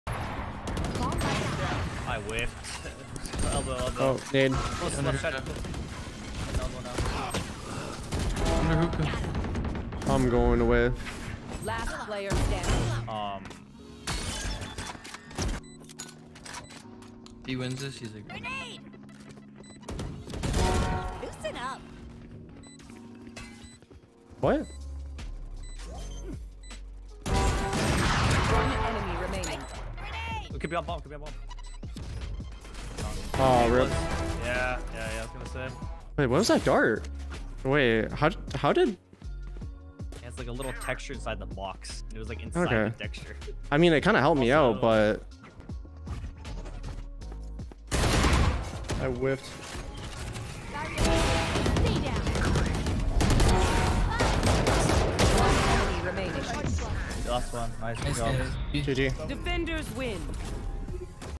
TenZ couldn't believe SOVA DART BYPASSES him & help him ACE
Enemy Sova uses his DART to detect TenZ but it fails. It actually stuck inside the small texture of the box and TenZ bypasses it. This helps TenZ to get the ACE but unlucky they lost the round. When TenZ says he whiff, don't trust it. He will repeek it and kill them all. POG

For any request or video that you want to be taken down